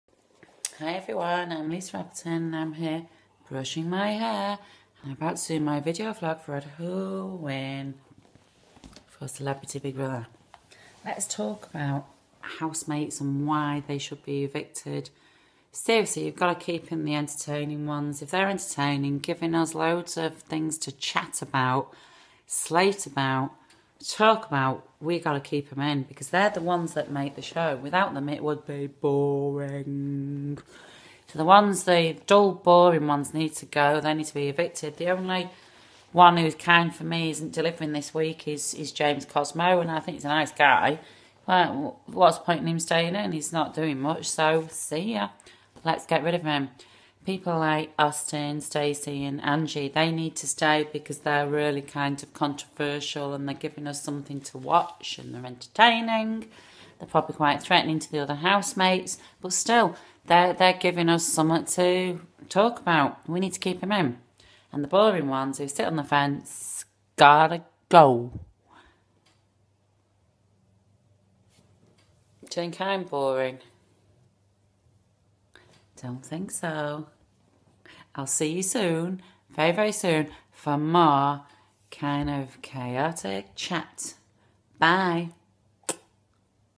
Lisa Appleton talks about why housemates should be evicted
Is time up for James Cosmo on Celebrity Big Brother?